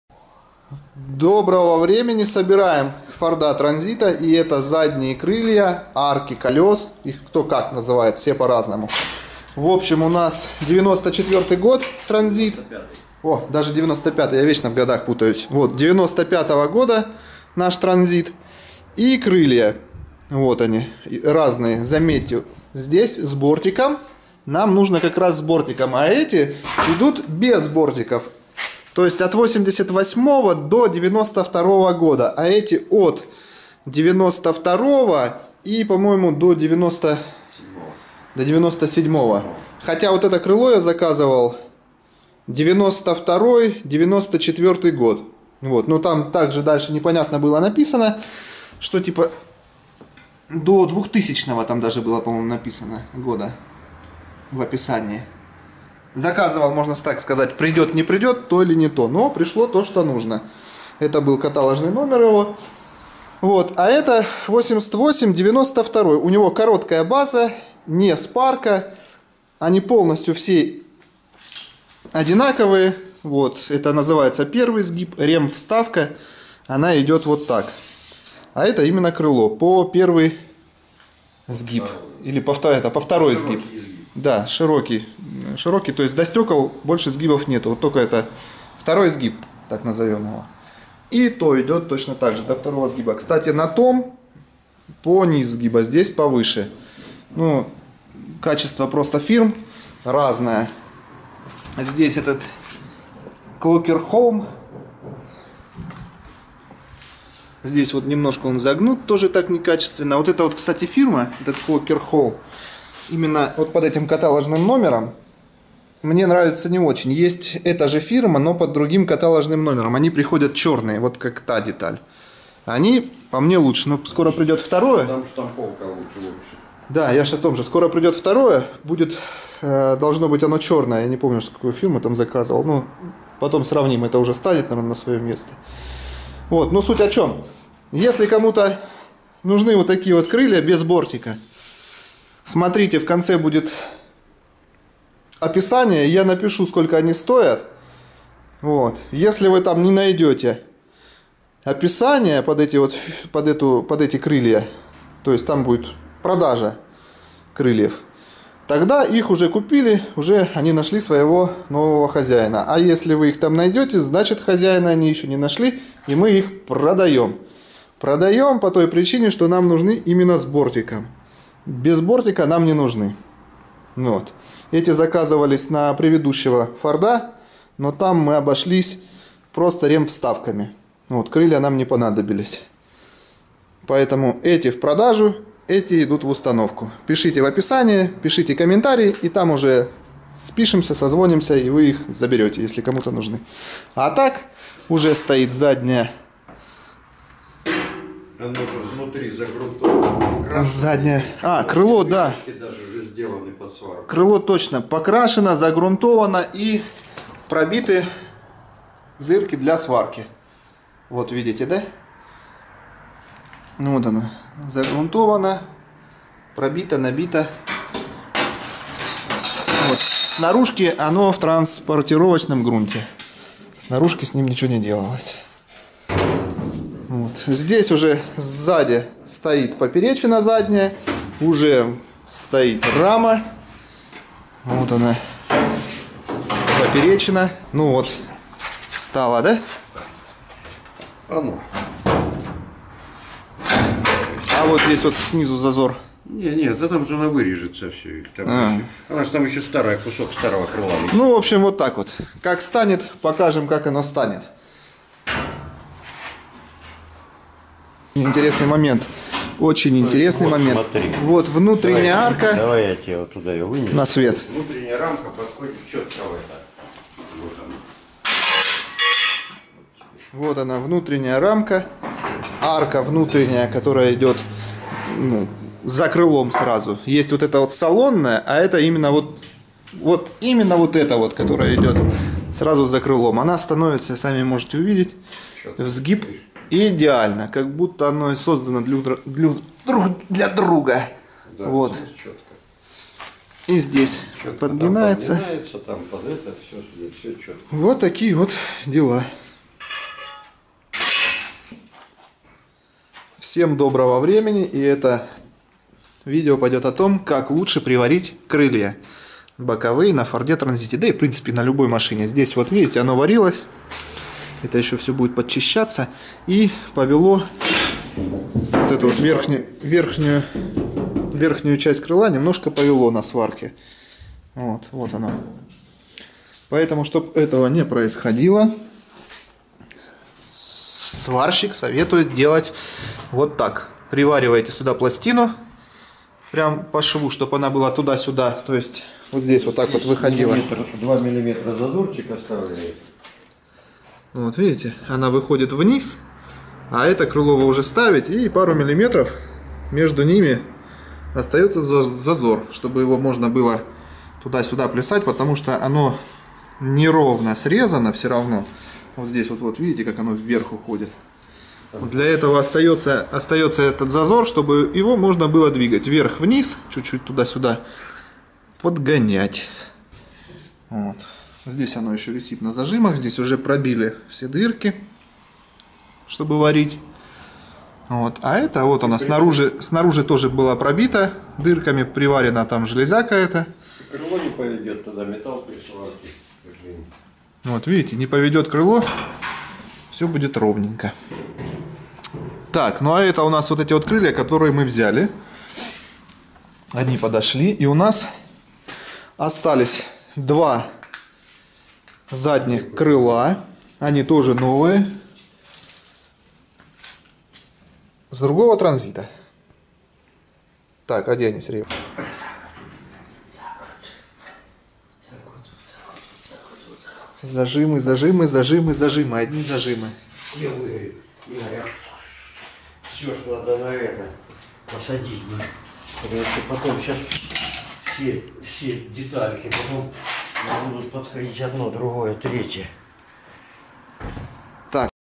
Запчасти. Задние крылья на Форд Транзит 95 год короткая база.
Остались у нас два новеньких и подготовленных крыла к установке на авто. Но нам они не понадобились и на этого форда не подошли. Были приобретены другие.
   Те которые мы продаем они идут на Форд Транзит до 91 года без бортика.
    1) POLCA  Польша ( кат номер 324484 )  TRANSIT 86-91 КРЫЛО ЗАДНЕЕ R 1/3 Это правое. его стоимость 2300. р
    2) Polcar Польша  ( кат номер 324483 ) Ford Transit / Форд Транзит 86-91 КРЫЛО ЗАДНЕЕ ЛЕВОЕ 1/3 Левое его стоимость 2300.р
 Мы использовали такие.
  1) KLOKKERHOLM Дания ( кат номер 2515531 )  Ford Transit / Форд Транзит 86-91 КРЫЛО ЗАДНЕЕ ЛЕВОЕ 1/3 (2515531) короткая база его стоимость 2300 но оно мне не понравилось потому как пришло оцинкованное ну и в общем не очень лучше взять такое ( 3245831 полкар)
   2) KLOKKERHOLM Дания ( кат номер 2515532 ) Ford Transit / Форд Транзит 86-91 КРЫЛО ЗАДНЕЕ ПРАВОЕ 1/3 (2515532) короткая база его стоимость 2460 оно мне тоже не понравилось.
 Если хотите приобрести те крылья которые мы продаем то пишите обменяемся контактами.